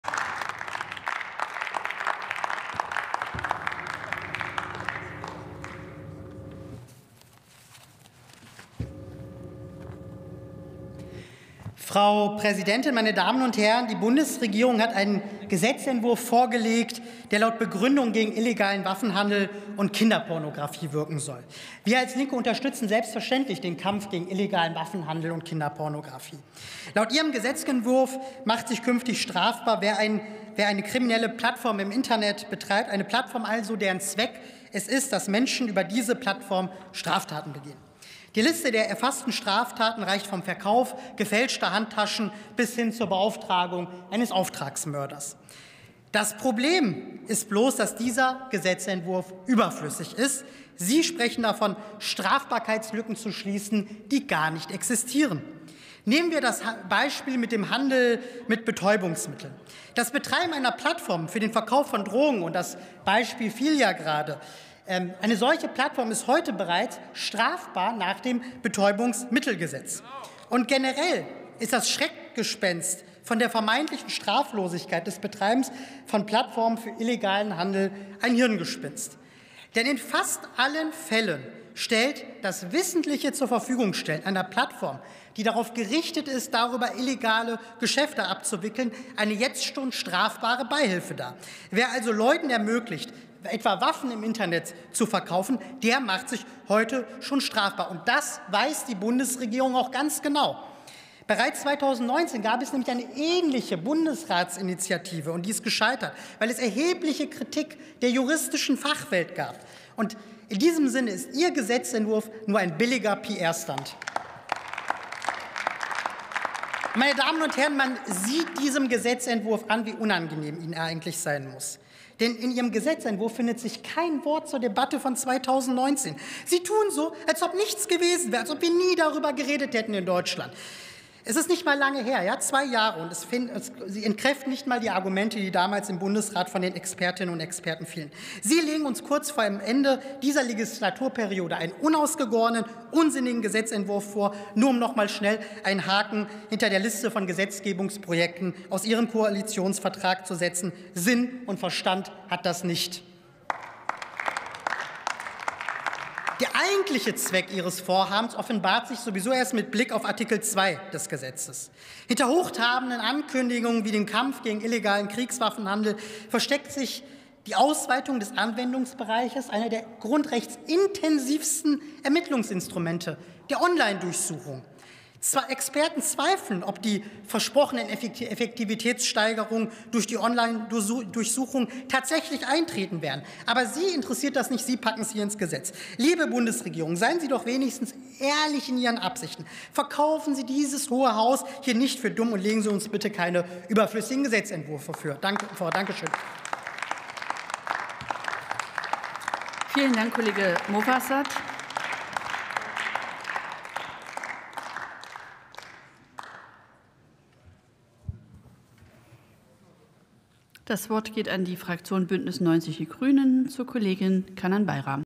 Niema Movassat, DIE LINKE: Strafrechtsverschärfung als PR-Stunt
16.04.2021 - Die Bundesregierung will das Betreiben krimineller Handelsplattformen unter Strafe stellen. Dabei ist die Zurverfügungstellung einer solchen Plattform heute schon über die Beihilfe-Regelung strafbar. In Wirklichkeit will die Regierung über die Hintertür die Online-Durchsuchung einzuführen. Sie kleidet diese Änderung in der Strafprozessordnung ein in angeblicher Bekämpfung von illegalem Waffenhandel und Kinderpornografie. Das ist schäbig und eine Irreführung.